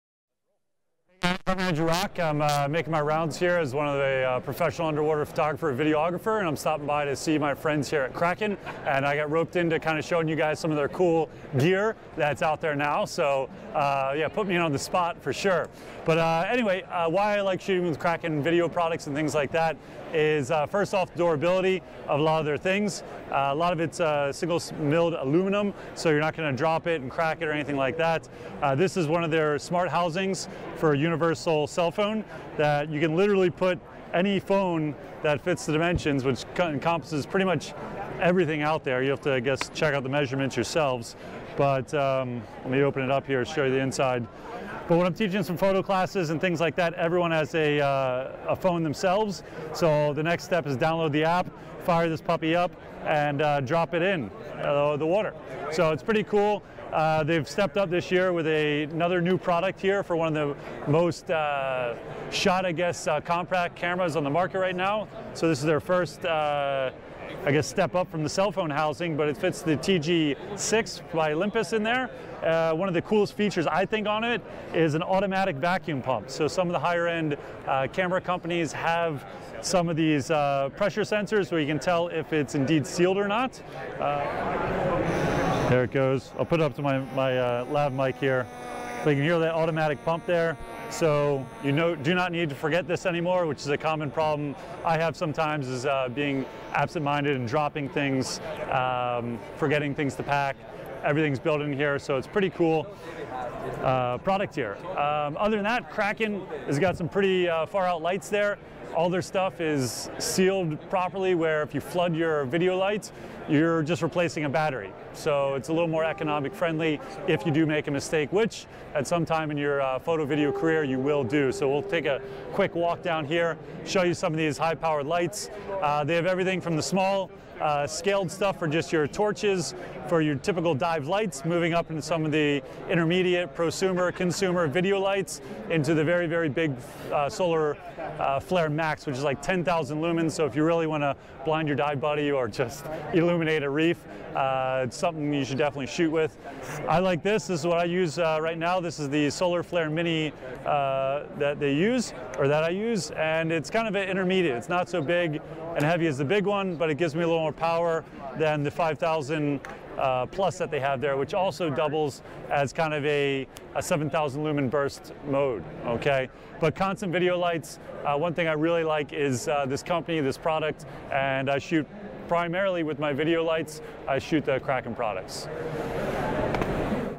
DEMA 2019 Coverage: Part 4 – Kraken Sports
DPG Editor Andrew Marriott reports from DEMA 2019—the industry’s biggest dive show. Here, he's hearing from Kraken Sports Fan Andrew Raak, who introduces the company's latest housings and video lights